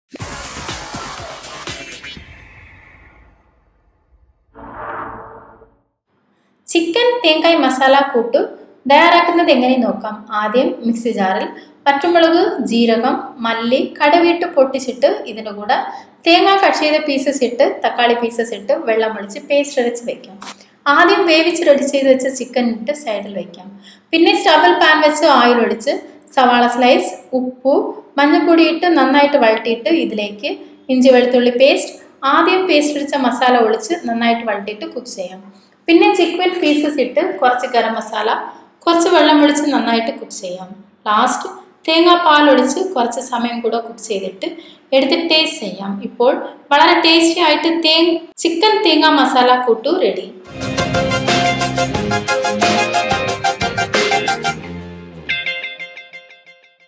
Chicken Thengai Masala Kottu / சிக்கன்தேங்காய்மசாலா - Tamil
A simple Tamil Nadu style chicken curry called as Kodi Kobbari Masala Tho Kura.

INGREDIENTS:

Dry red chilies -  4 number
Cumin seeds -  1 tea spoon
Coriander seeds -  1 tea spoon
Mustard seeds -  1 tea spoon
Fresh coconut (Pieces) -  ½ cup
Tomato (Chopped) -  2 number
Boiled chicken -  1 cup
Oil  -  as required
Onion (Chopped) -  1 number
Ginger garlic paste -  1 tea spoon
Garam masala powder -  1 tea spoon

Directions:

Dry roast the dry red chilies, cumin seeds, coriander seeds, mustard seeds and transfer into a blender and later add fresh coconut pieces, tomato and make a fine paste and keep aside.

In a pan add oil, onion, salt, turmeric powder, ginger garlic paste, earlier prepared paste and saute it.

Add boiled chicken, garam masala powder, water, coconut milk and cook it well.

Serve it with hot rice.